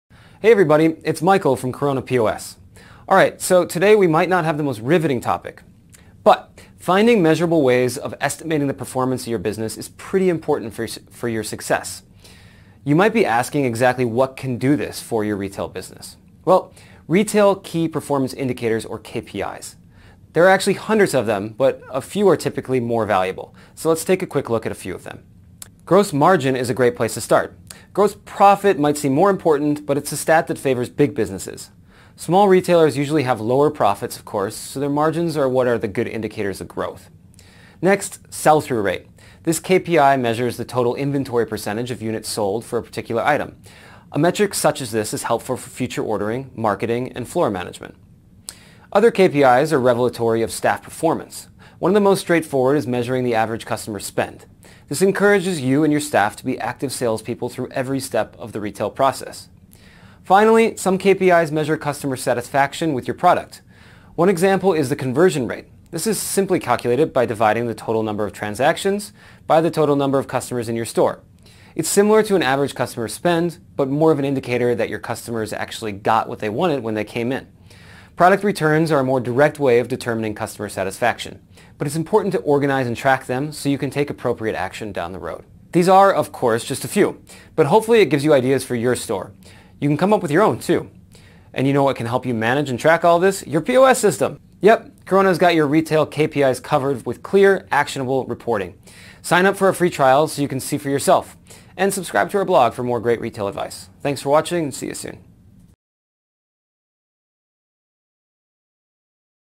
Most Important Retail KPIs
Having a great POS system with advanced reporting and metrics is great. But you need to know how to break down and process the data.